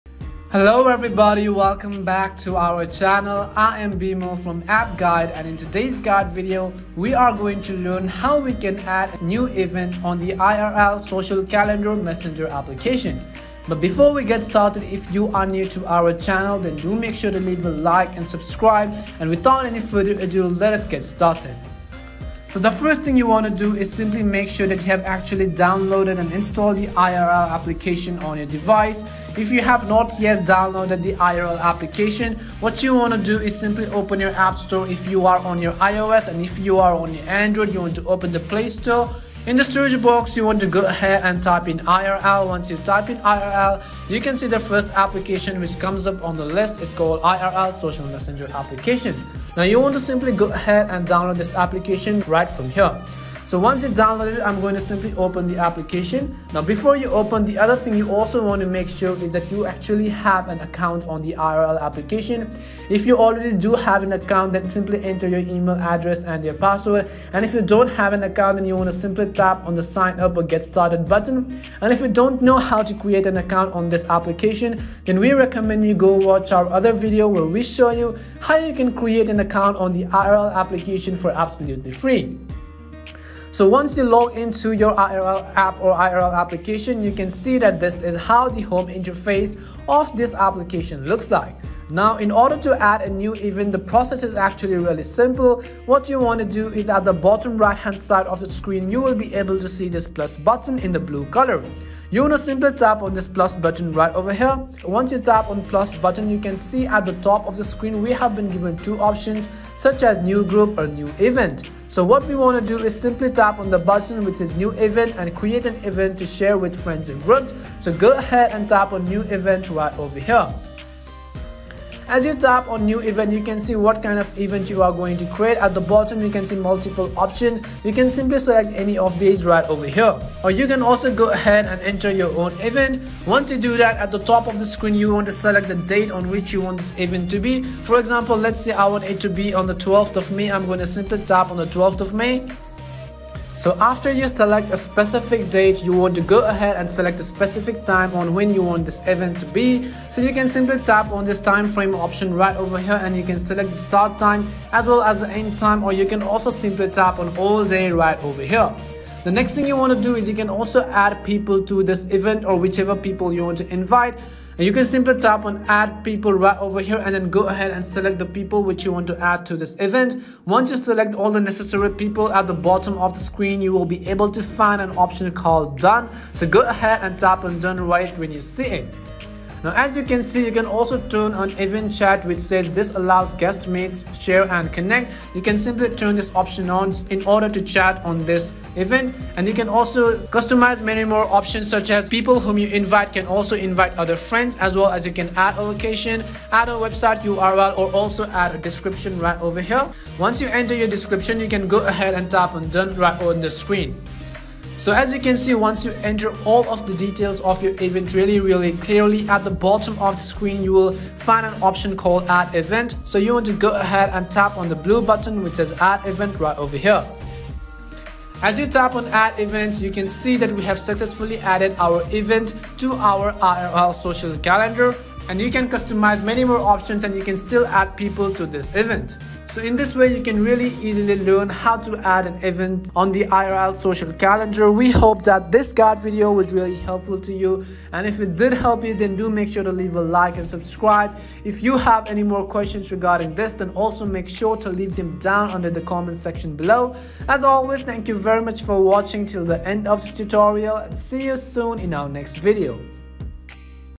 How to Add Event to IRL Social Calendar 2022
Step 1: Make sure you have downloaded and installed the IRL Application from your devices respective App Store
Step 2: Once you download it, go ahead and open the application
Step 3: Log in to your IRL Account.
Step 4: If you don't have an account, simply tap on Register and create a new account
Step 5: After you log in, tap on the Plus + icon at the bottom right of the screen
Step 6: At the top, select New Event
Step 7: Fill in all the details and tap on Add Event button at the bottom.

~ Time Stamps:
0:00 Introduction
0:22 How to Download IRL App
0:53 Login to IRL
1:07 Sign Up on IRL
1:12 How to Create an IRL Account
1:22 How to Add Event to IRL Calendar
2:00 Select Event Name
2:13 Select Event Date
2:23 Select Event Time
2:39 Add People to the Event
2:59 Other Event Details
3:18 Add Event Description
3:37 Add Event Success!
3:49 Outro: Final Verdict